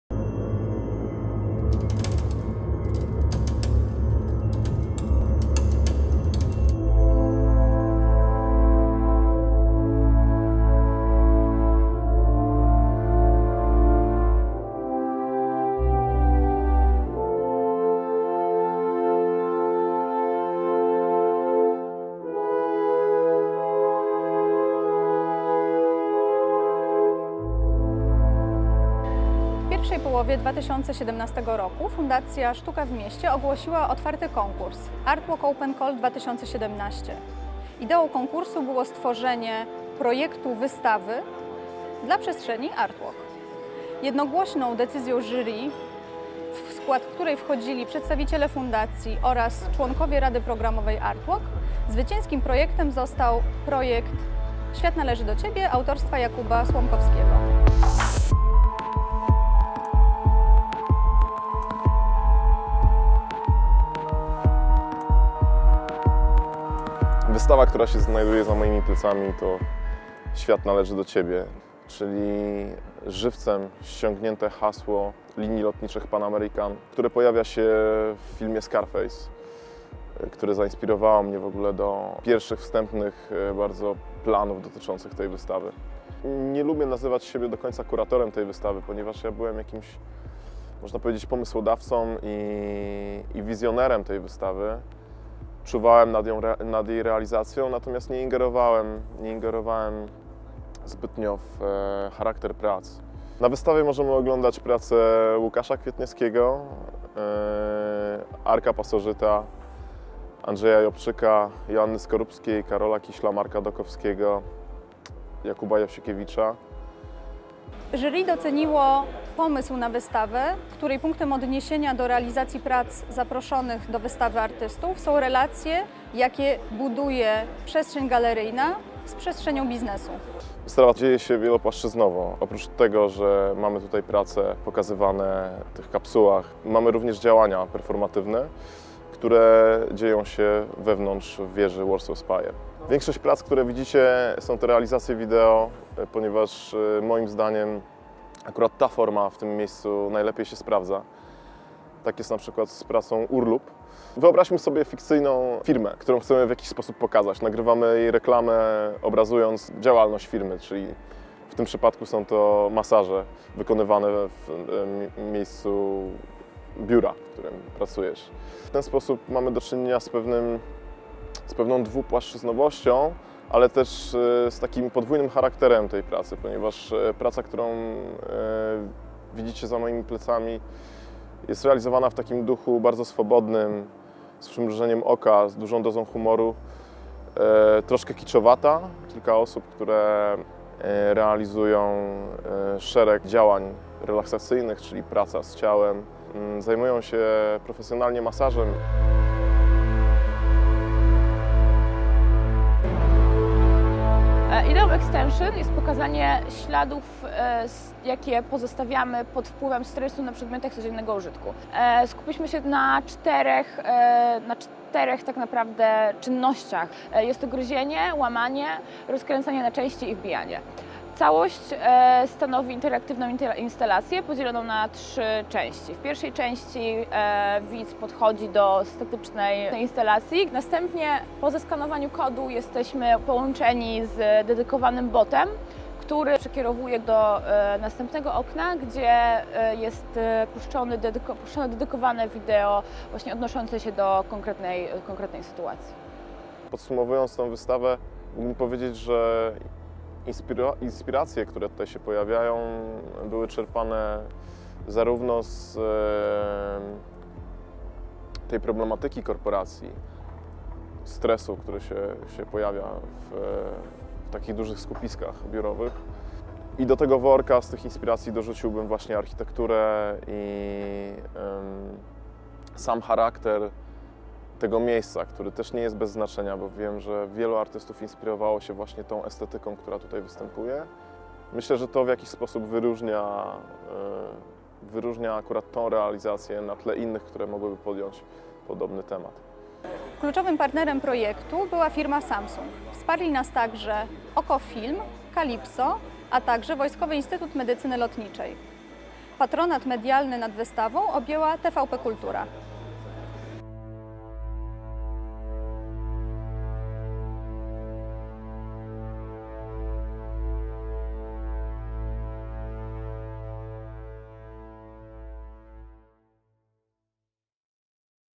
Art Walk. Wystawa "Świat należy do Ciebie"
W pierwszej połowie 2017 roku fundacja Sztuka w Mieście we współpracy z Ghelamco Poland ogłosiła otwarty konkurs na opracowanie projektu artystycznego – koncepcji wystawy dla galerii Art Walk zlokalizowanej na placu Europejskim w Warszawie (Art Walk Open Call 2017). Jednogłośną decyzją jury zwycięską pracą został projekt „Świat należy do Ciebie” autorstwa Jakuba Słomkowskiego. Jury doceniło pomysł na wystawę, którego punktem odniesienia do realizacji prac przez zaproszoną grupę artystów są fizyczne, funkcjonalne i symboliczne relacje, jakie galeria buduje pomiędzy światem sztuki a przestrzenią biznesu.

Istotą wystawy Świat należy do Ciebie jest zdefiniowanie na nowo przestrzeni korporacyjnej, przy wykorzystaniu narzędzi, którymi posługują się zarówno artyści, jak i świat reklamy.
Realizacje artystów są nowymi propozycjami produktów, usług czy też wizji, których naturalnym źródłem są przestrzenie oraz relacje biurowe. Wystawa Świat należy do Ciebie ma charakter procesu podczas którego część artystów przeprowadzi swoje działania w ciągu trzech miesięcy trwania ekspozycji.

Zaproszeni do realizacji przedsięwzięcia artyści mają do dyspozycji 20 „okien” wystawienniczych, a także przestrzeń na terenie pobliskiego wieżowca Warsaw Spire. W zainteresowaniu wszystkich uczestników główną rolę odgrywa kontekst miejsca i wykorzystanie możliwości, jakie daje zastana lokalizacja. Być może dzięki niej zaistnieje nowa sieć powiązań i współpracy, podobna do ciała korporacyjnego, której siłą napędową będzie jednak refleksja. Ekspozycja jest także przyczynkiem do dyskusji na temat kondycji artysty balansującego między światem sztuk wizualnych, a światem komercji.

kurator: Jakub Słomkowski

koordynatorka projektu: Karolina Wlazło-Malinowska

artyści: Arek Pasożyt, Jakub Jasiukiewicz, Andrzej Jobczyk, Marek Dakowski, Karol Kisiel, Łukasz Kwietniewski, Jakub Słomkowski, Radicalzz: Joanna Skorupska, Marta Frączek

partnerzy: Samsung, Calypso, O.K.O Film, Wojskowy Instytut Medycyny Lotniczej

patronat medialny: TVP Kultura

współpraca: Katarzyna Rysiak, Bakcyl Studio, Iza Szostak, Tomasz Pakowski, Daniel Truszkowski, Karolina Ogrodowska, Gilberto Bautista Ortiz, Marcin Fijałkowski, Jakub Dykiert, Dana Chmielewska, Agnieszka Sprawka, Bakcyl Studio, Zofia Turkot, Marek Kucharski, Michał Rytel-Przełomiec, Karolina Karwan

organizator: Fundacja Sztuka w Mieście, Ghelamco Poland
miejsce: Art Walk, plac Europejski, Warszawa